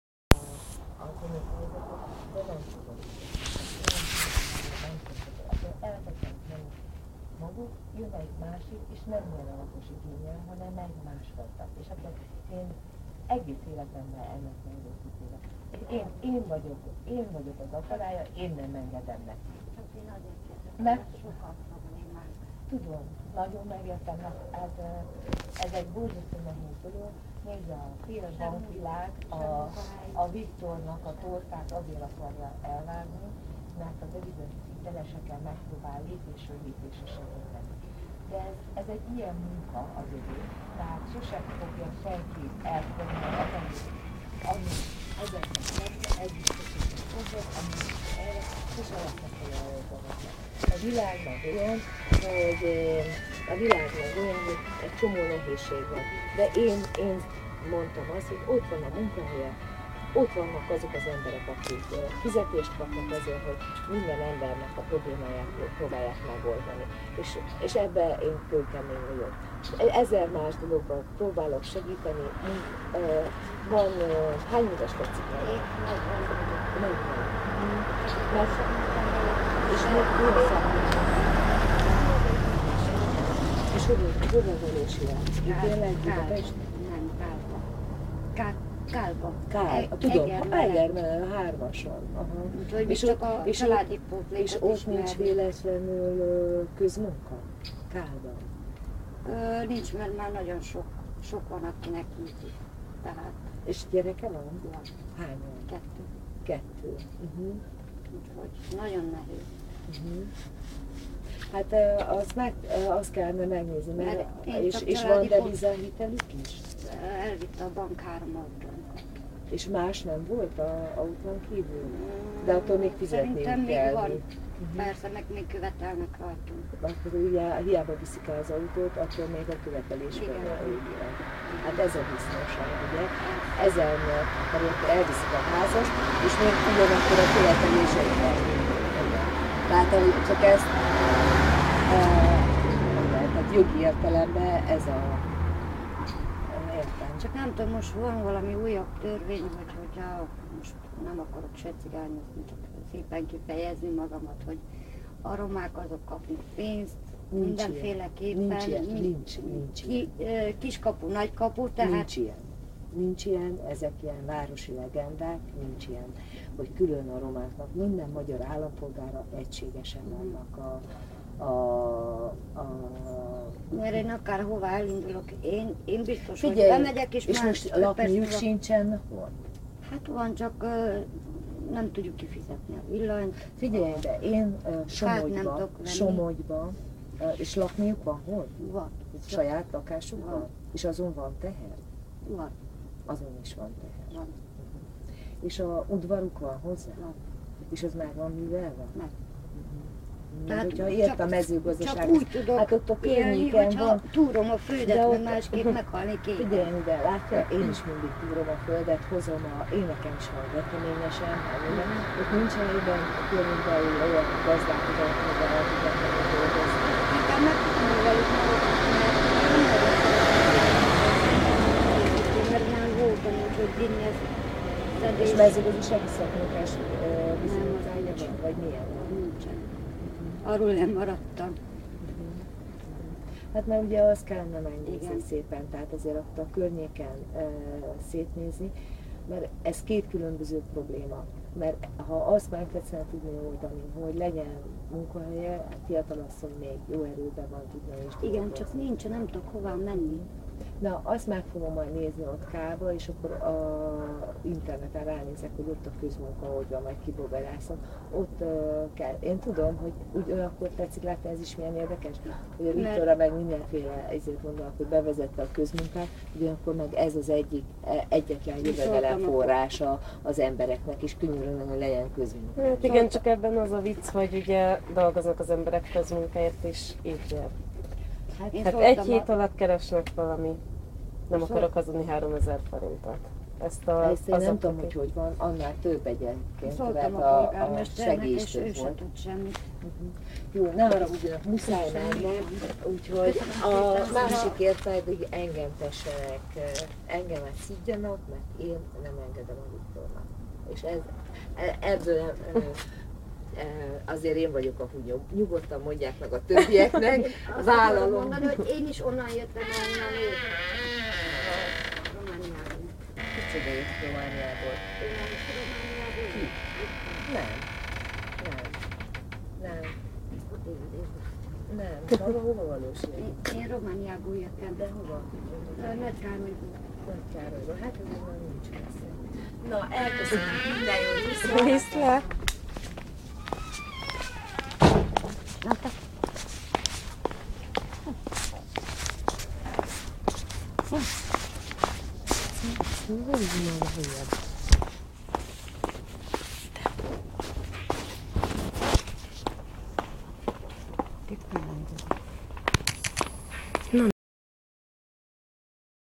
2013.09.05 Orbán éhségsztrájk - Lévai kormányoz
Kiderült, hogy Lévai Anikó kormányozza az országot, nem a parlament, nem Viktor, hanem Anikó. Az történik, amit Anikó mond! Anikó meglepődött, amikor a hátsó bejáratnál megjelentek az aktivisták.